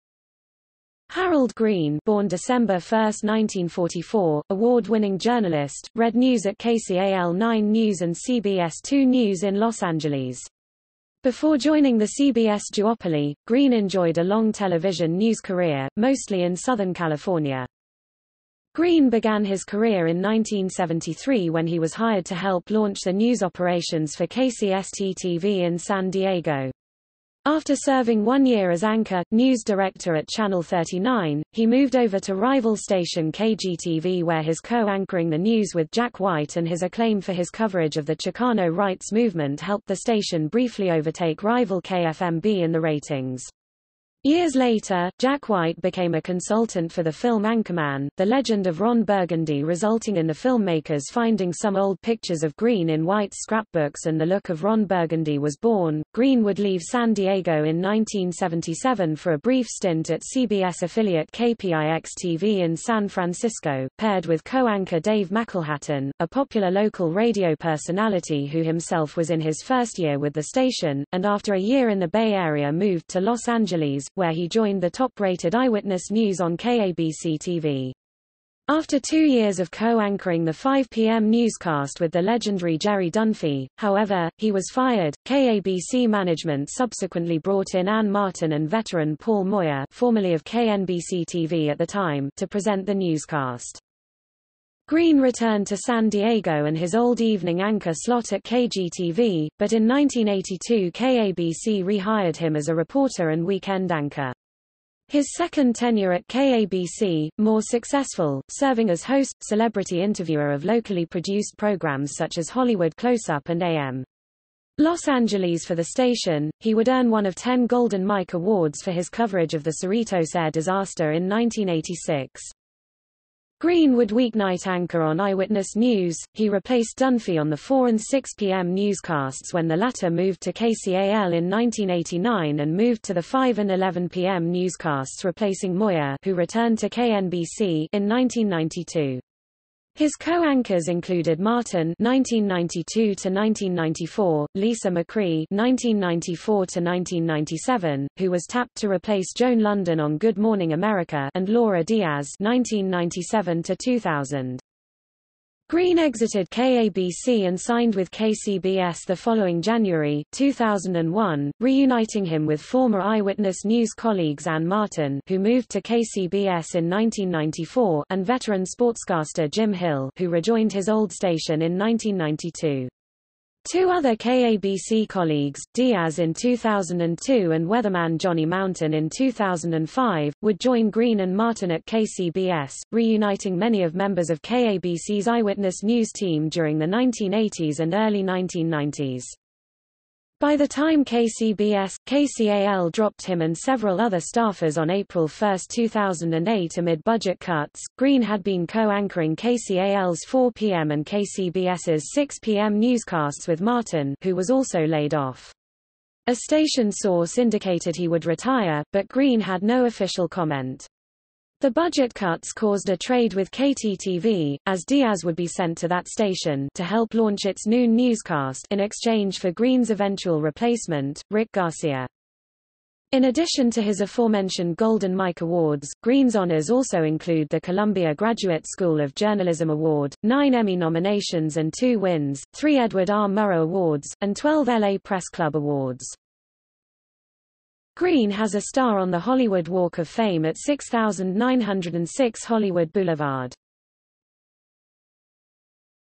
Harold Greene (journalist) | Wikipedia audio article
- Socrates

SUMMARY
Harold Greene (born December 1, 1944), award-winning journalist, read news at KCAL 9 News and CBS 2 News in Los Angeles.  Before joining the CBS duopoly, Greene enjoyed a long television news career, mostly in Southern California.
Greene began his career in 1973 when he was hired to help launch the news operations for KCST-TV in San Diego.  After serving one year as anchor/news director at Channel 39, he moved over to rival station KGTV where his co-anchoring The News with Jack White and his acclaim for his coverage of the Chicano rights movement helped the station briefly overtake rival KFMB in the ratings.  Years later, Jack White became a consultant for the film Anchorman: The Legend of Ron Burgundy resulting in the filmmakers finding some old pictures of Greene in White's scrapbooks and the look of Ron Burgundy was born.Greene would leave San Diego in 1977 for a brief stint at CBS affiliate KPIX-TV in San Francisco, paired with co-anchor Dave McElhatton, a popular local radio personality who himself was in his first year with the station, and after a year in the Bay Area moved to Los Angeles, where he joined the top-rated Eyewitness News on KABC-TV.  After two years of co-anchoring the 5:00pm newscast with the legendary Jerry Dunphy, however, he was fired; KABC management subsequently brought in Ann Martin and veteran Paul Moyer (formerly of KNBC-TV at the time) to present the newscast.
Greene returned to San Diego and his old evening anchor slot at KGTV, but in 1982 KABC rehired him as a reporter and weekend anchor.  His second tenure at KABC, more successful; serving as host/celebrity interviewer of locally produced programs such as Hollywood Close-Up and A.M. Los Angeles for the station, he would earn one of ten Golden Mike Awards for his coverage of the Cerritos air disaster in 1986.
Greene would weeknight anchor on Eyewitness News - he replaced Dunphy on the 4 and 6 p.m. newscasts when the latter moved to KCAL in 1989 and moved to the 5 and 11 p.m. newscasts replacing Moyer (who returned to KNBC) in 1992.  His co-anchors included Martin (1992–1994), Lisa McRee (1994–1997, who was tapped to replace Joan Lunden on Good Morning America) and Laura Diaz (1997–2000).
Greene exited KABC and signed with KCBS the following January,  2001, reuniting him with former Eyewitness News colleagues Ann Martin (who moved to KCBS in 1994) and veteran sportscaster Jim Hill (who re-joined his old station in 1992). Two other KABC colleagues, Diaz in 2002 and weatherman Johnny Mountain in 2005, would join Greene and Martin at KCBS, reuniting many of members of KABC's Eyewitness News team during the 1980s and early 1990s.
By the time KCBS/KCAL dropped him and several other staffers on April 1, 2008 amid budget cuts, Greene had been co-anchoring KCAL's 4pm and KCBS's 6pm newscasts with Martin (who was also laid off). A station source indicated he would retire, but Greene had no official comment.  The budget cuts caused a trade with KTTV, as Diaz would be sent to that station (to help launch its noon newscast) in exchange for Greene's eventual replacement, Rick Garcia.
In addition to his aforementioned Golden Mike awards, Greene's honors also include the Columbia Graduate School of Journalism Award, nine Emmy nominations and two wins, three Edward R. Murrow Awards, and twelve L.A. Press Club Awards.
Greene has a star on the Hollywood Walk of Fame at 6906 Hollywood Blvd.